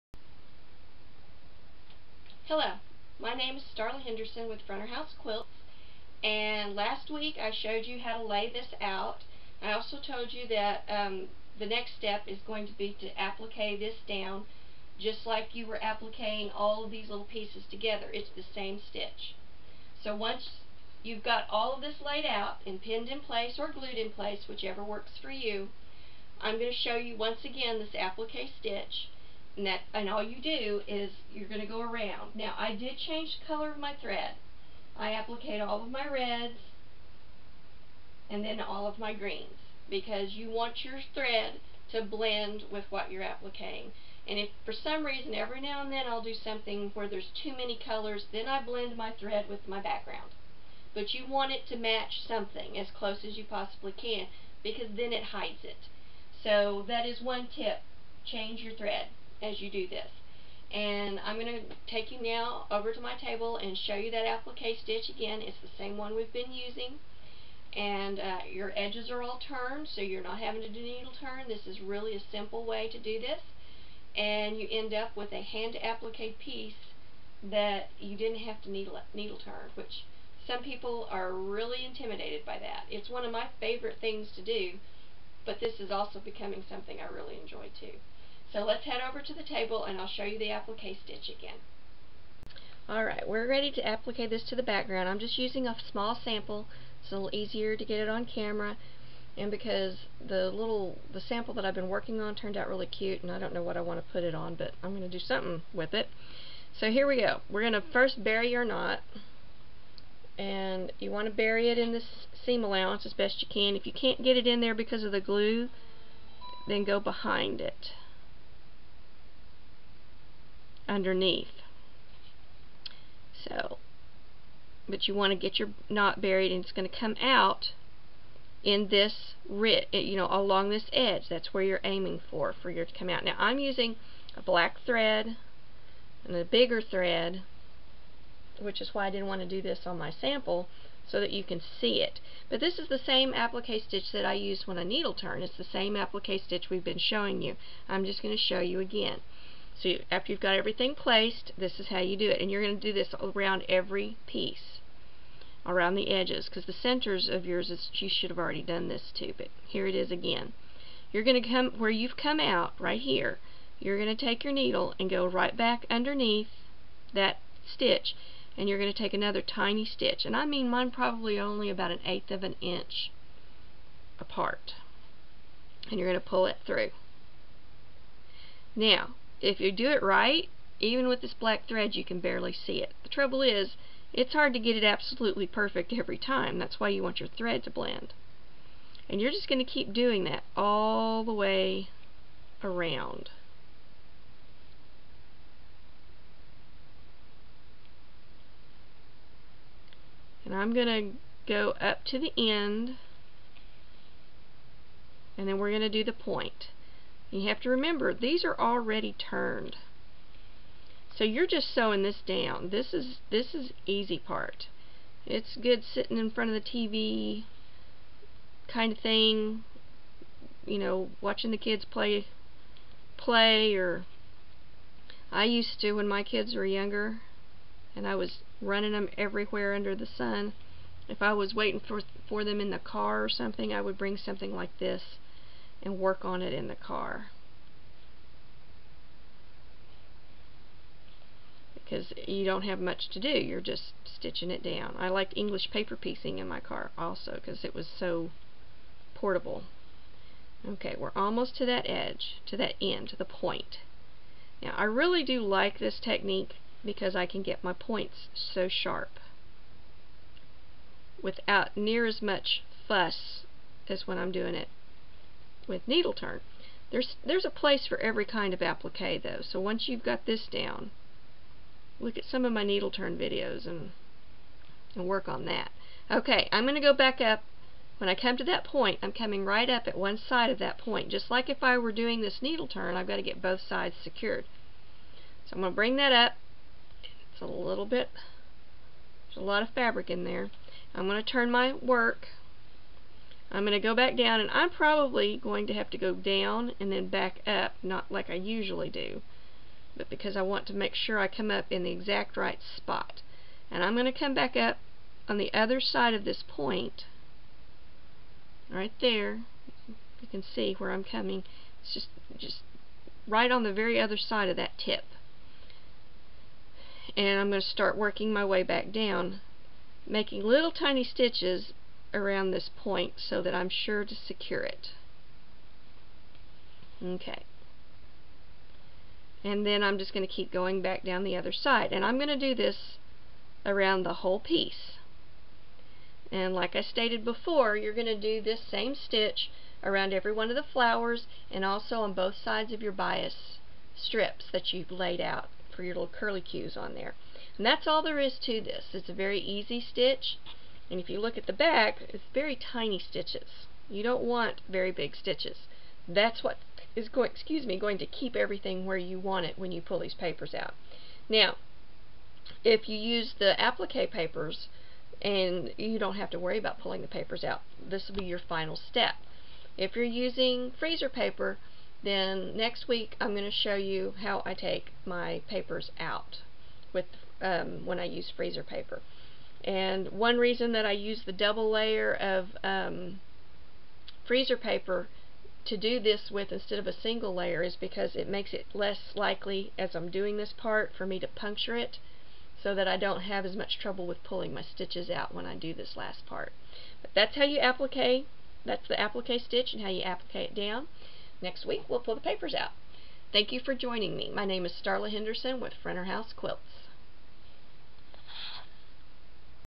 Winter Sampler 8: How To Applique Onto Your Background
This video will show you how I applique my pieces to the background fabric.